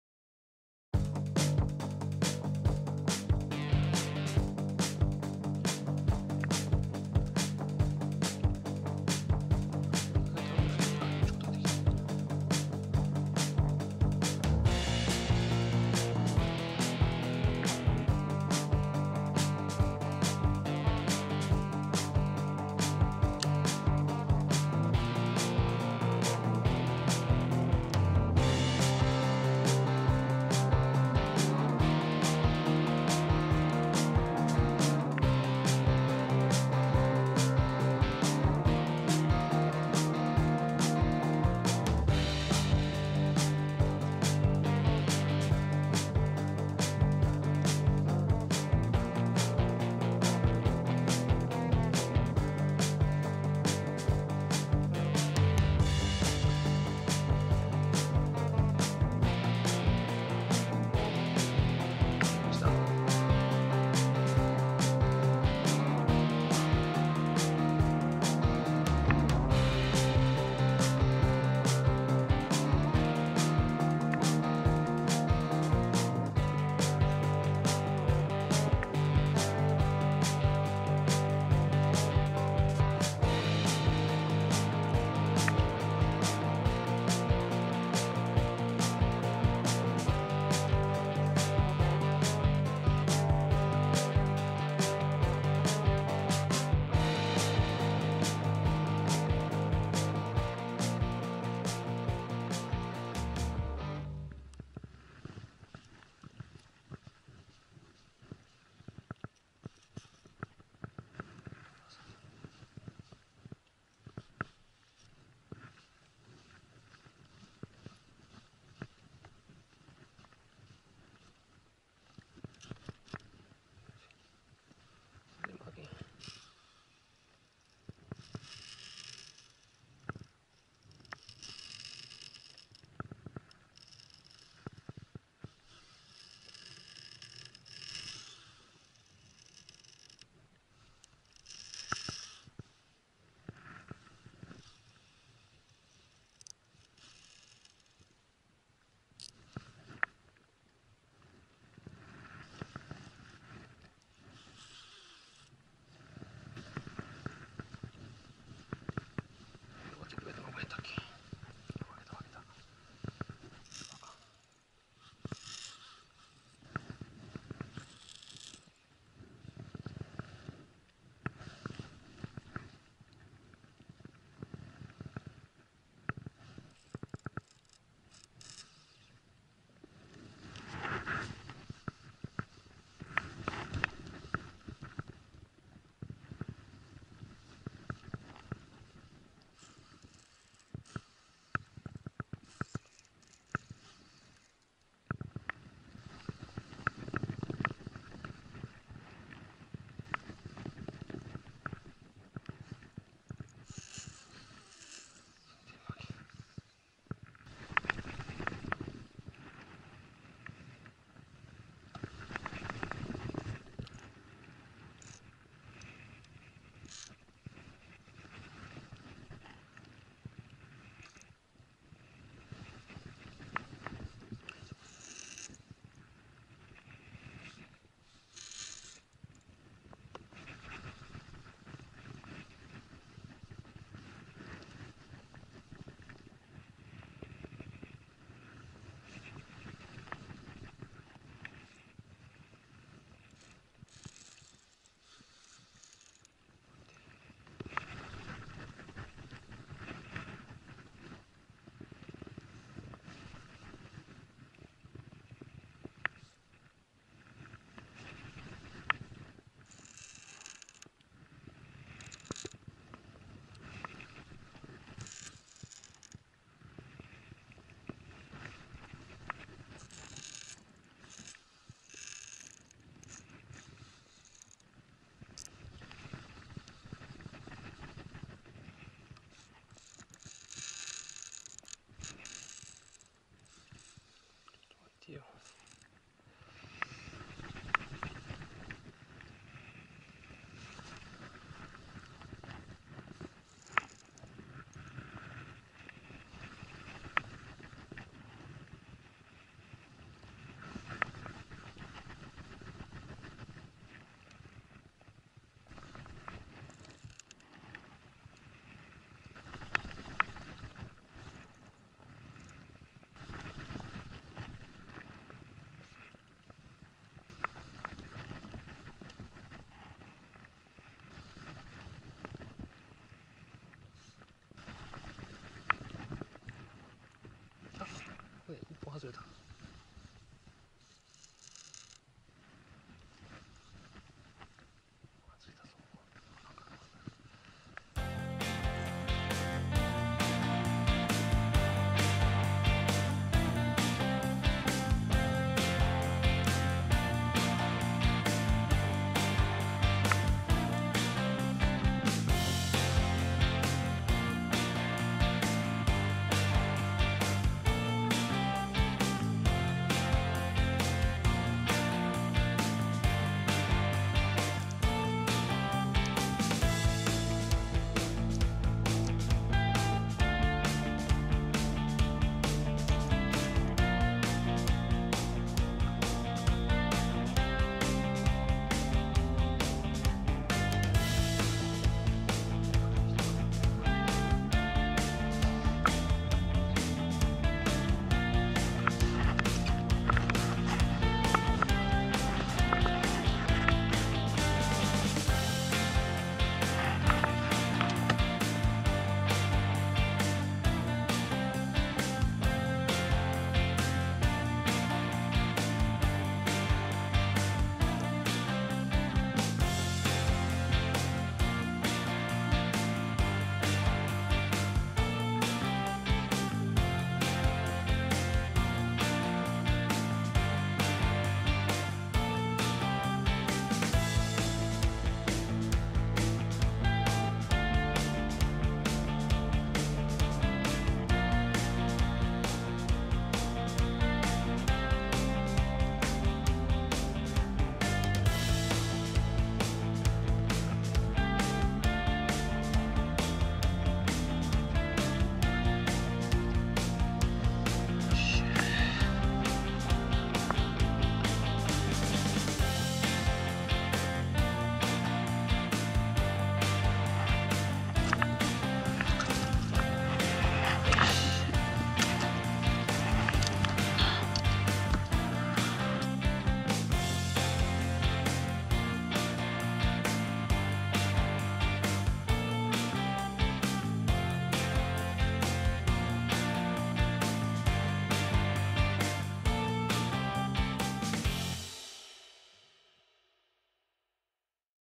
真冬の厳しい海でタイラバで大鯛キャッチ成功 Big snapper 68cm
海の中は真冬、雪代も入って非常に厳しい中、ほぼ丸坊主状態の中、最後の最後に大鯛がヒット。一進一退のファイトの末、無事キャッチ成功。うれしい1枚です。